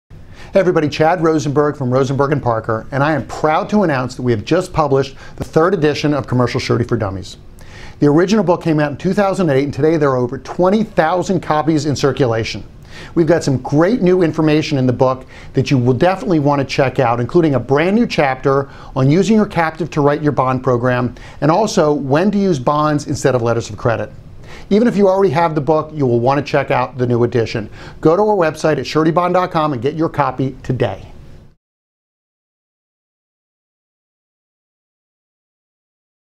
Commercial Surety for Dummies - Third Edition
Hi everybody, Chad Rosenberg from Rosenberg & Parker and I am proud to announce that we have just published the third edition of commercial surety for dummies.  The original came out 2008, today there are over 20,000 copies in circulation.  We've got some great new information in the book that you will want to check out. Including a brand-new chapter on using a captive to write your bond program and also when to use bonds instead of letters of credit.  Even if you already have the book you'll want to check out the new addition.  Go to our website at suretybond.com and get your
copy today.